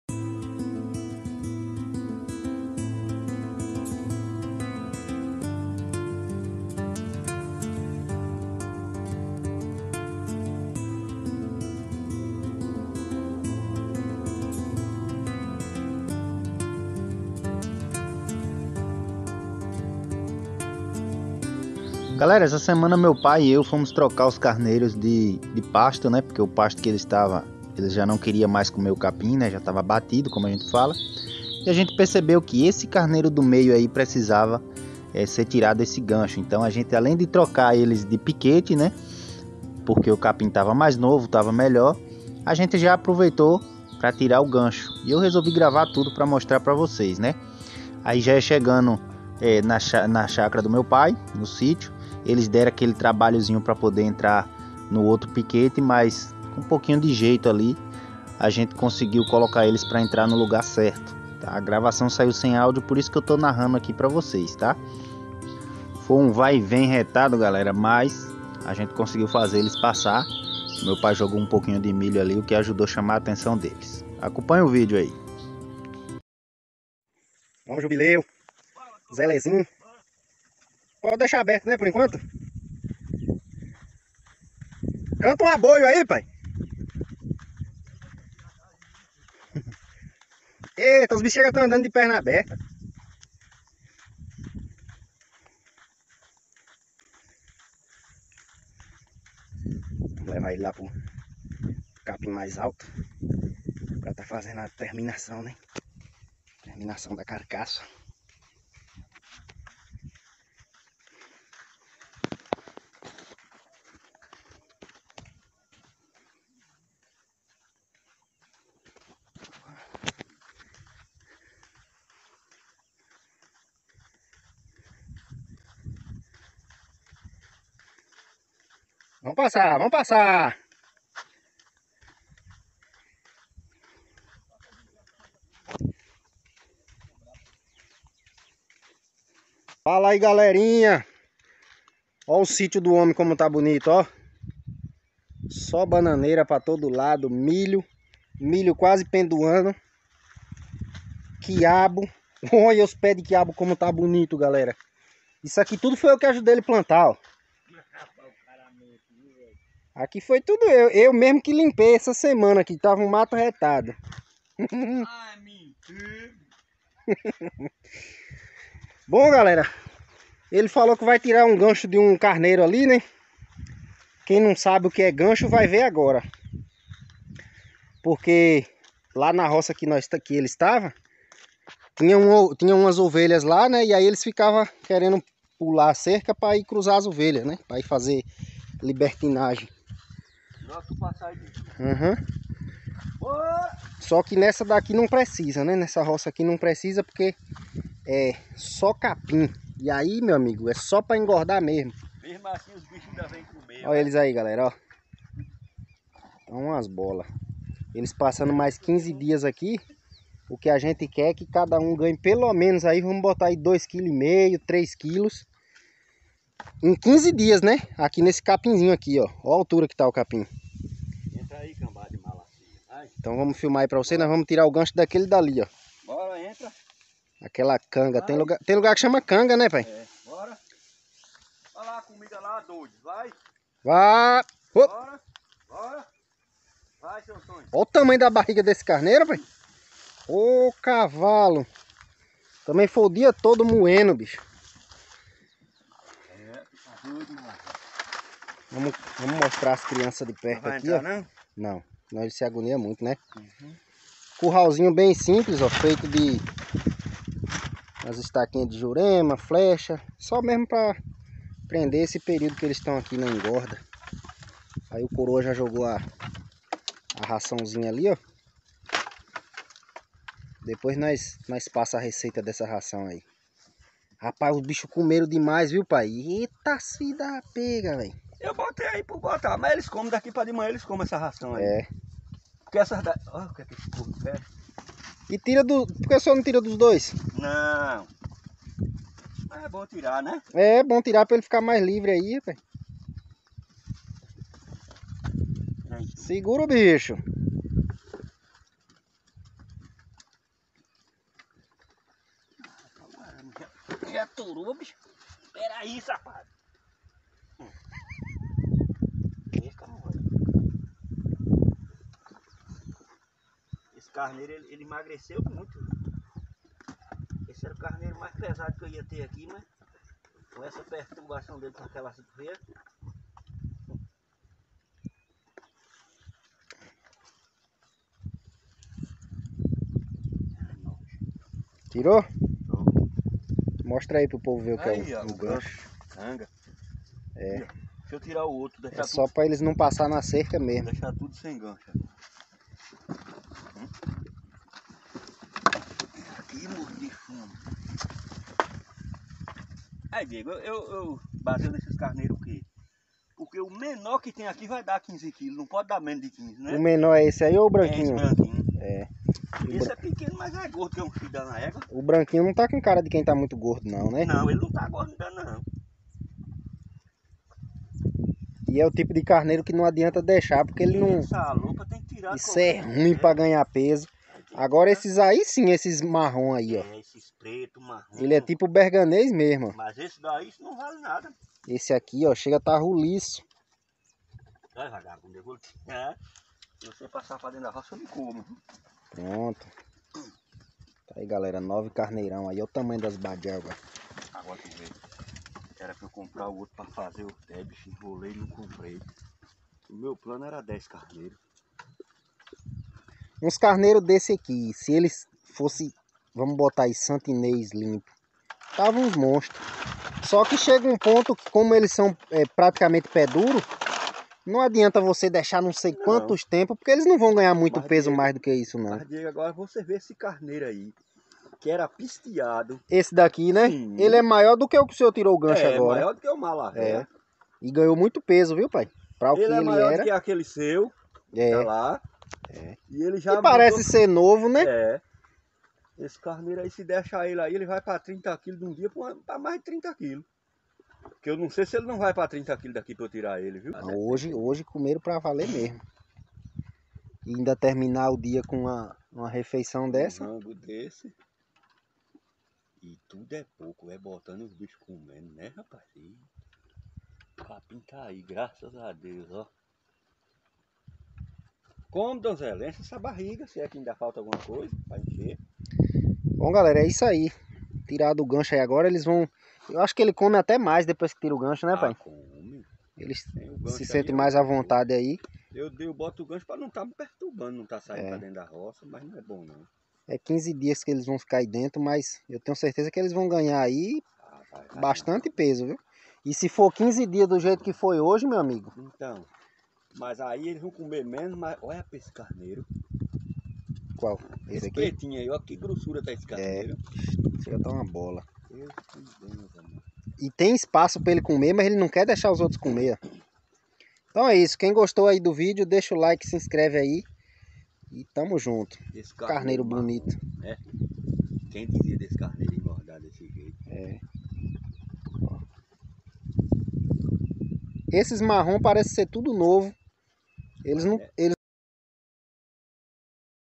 Tivemos que prender os carneiros para retirar o GANCHO de um deles
Fala galera nesse vídeo vamos trocar todos os carneiros de piquete e aproveitar para prender no curral e retirar o gancho de um deles.
O gancho e um pedaço madeira em forma de Y que é colocado no pescoço do animal evitando assim que ele passe pela cerca de arame farpado.